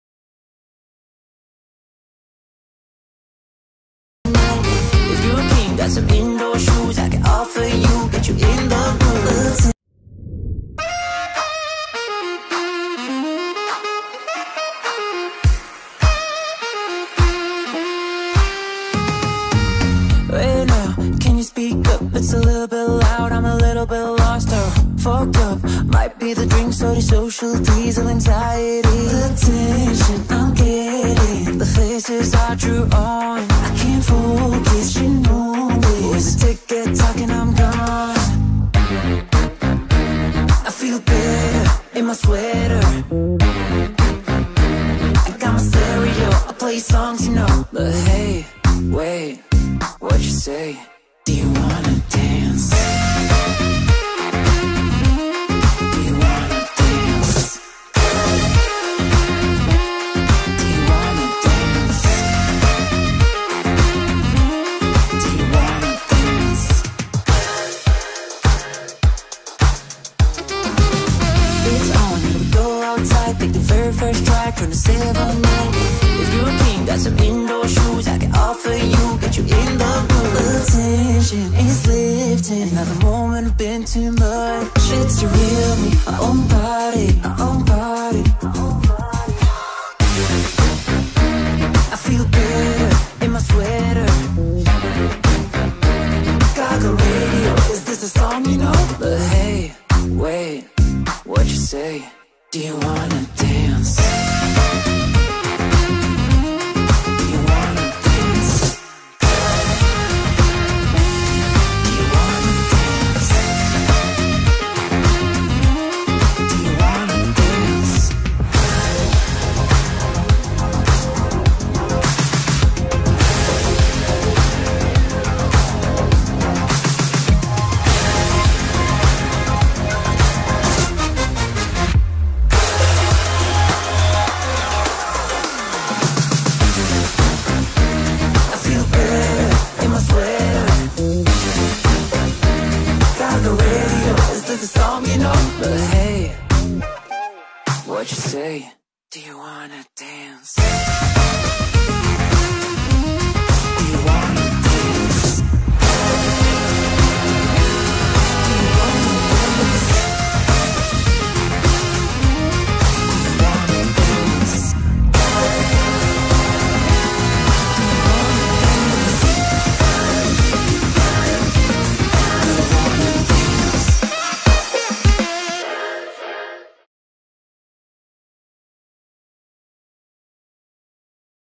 OUR OWN PARTY LINE DANCE | DOUBLE M STUDIO | Choreo by Roy Hadisubroto, Shane McKeever, Jo Thompson
Choreographer: Roy Hadisubroto (NL), Shane McKeever (N.IRE) & Jo Thompson Szymanski (USA) - October 2023
Demo by Gang's Jumat